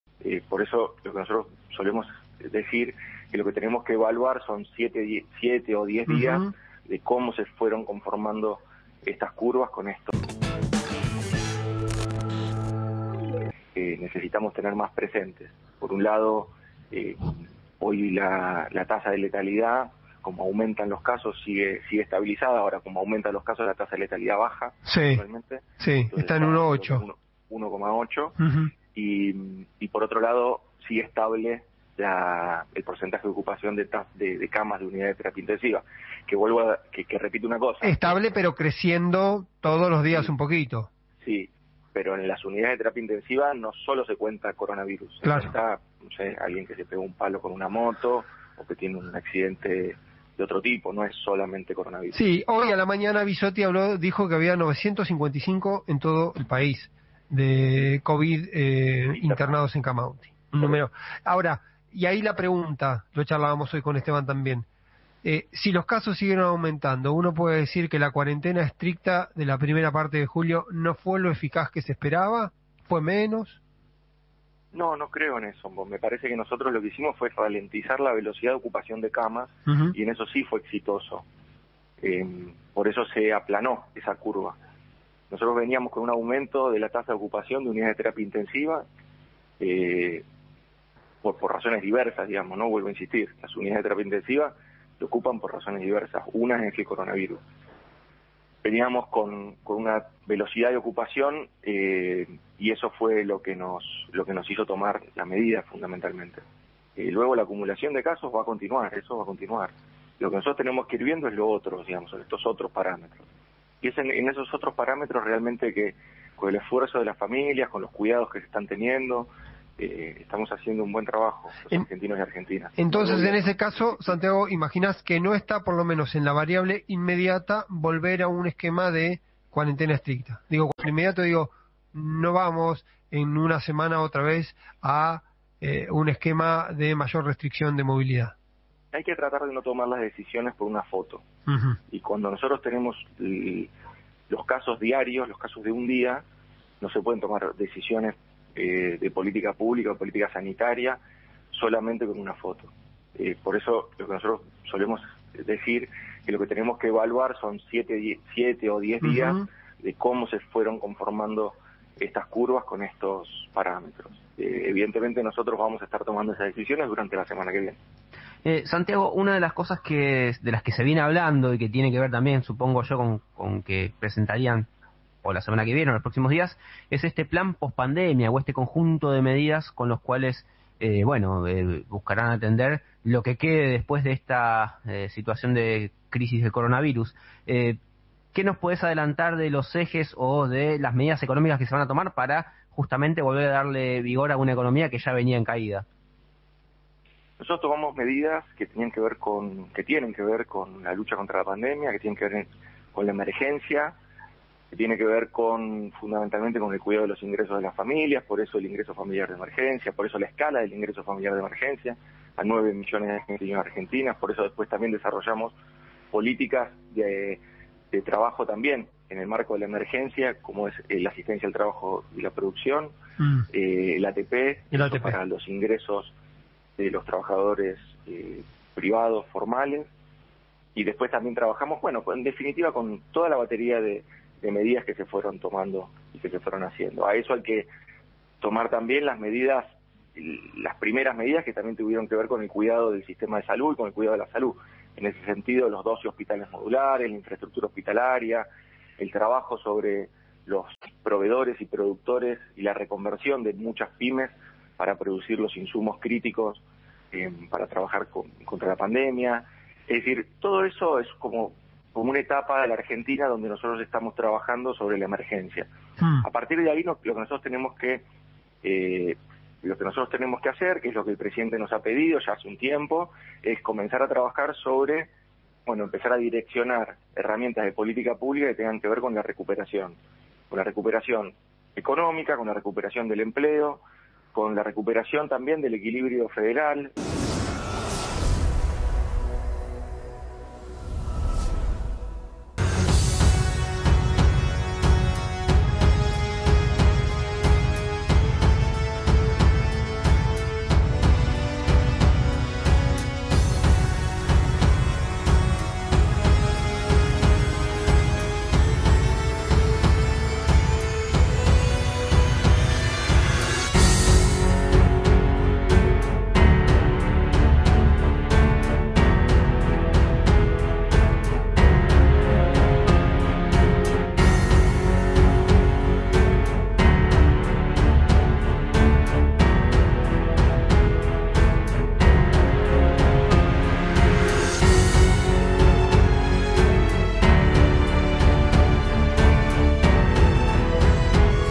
Cafiero "No hay que tomar decisiones con una foto, sino evaluar un período de 7 o 10 días"
El jefe de Gabinete afirmó hoy que la tasa de letalidad por coronavirus "está en baja" y el porcentaje de ocupación de camas de terapia intensiva está "estabilizado".

ID6